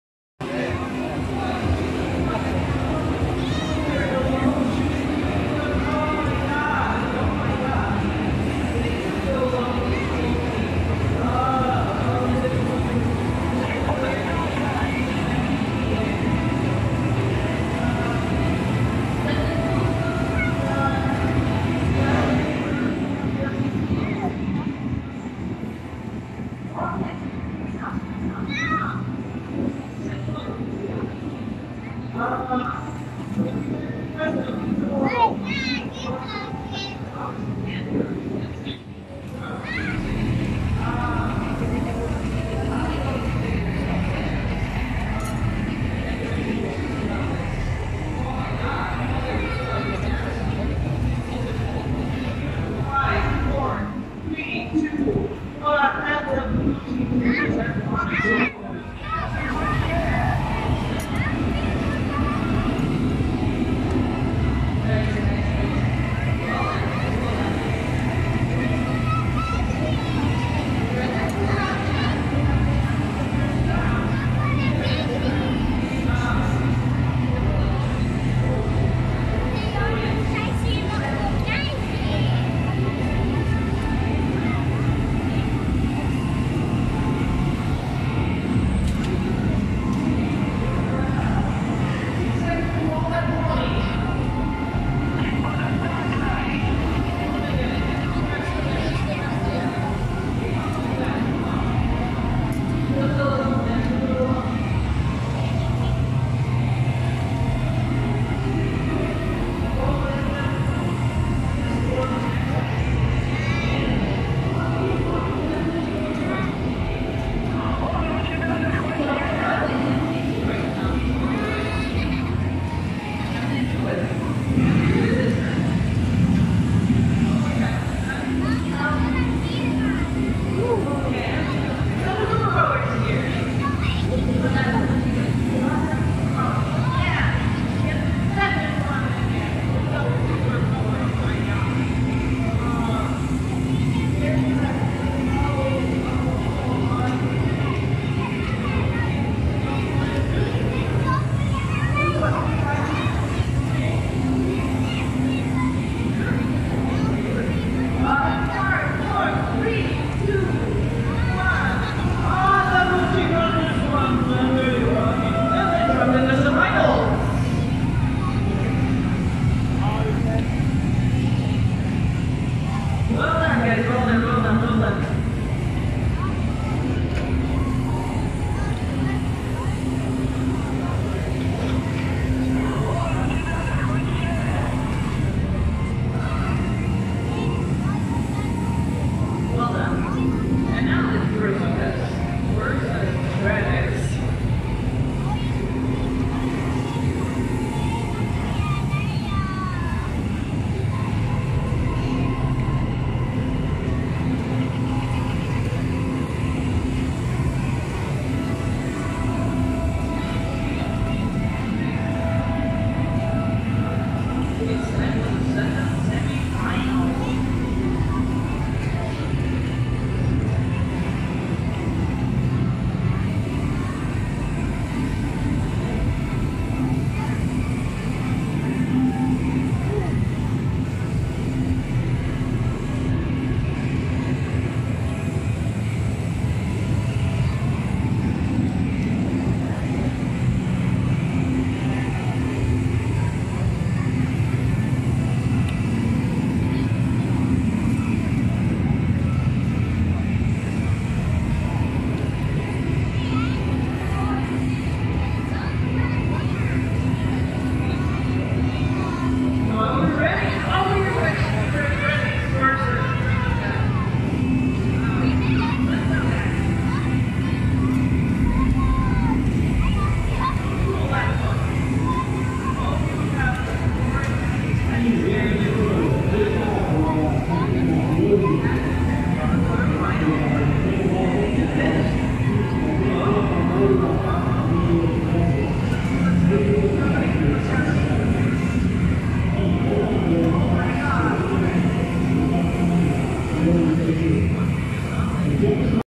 FLight simulator test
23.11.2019 in Hypertown, Tallinn. First time in the flight simulator test.